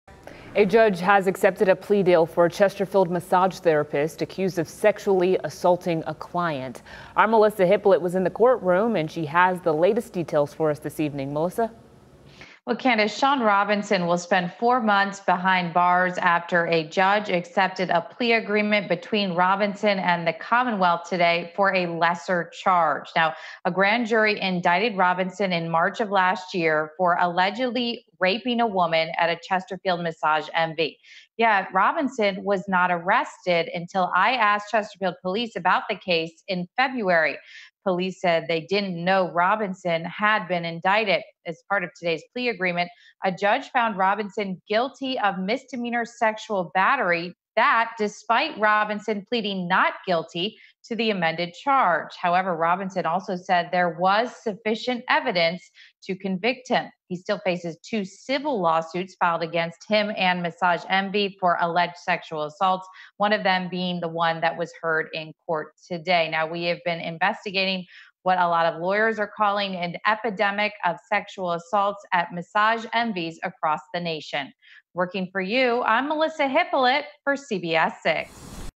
Massage Envy assault update
Former massage therapist at Massage Envy found guilty of sexual battery.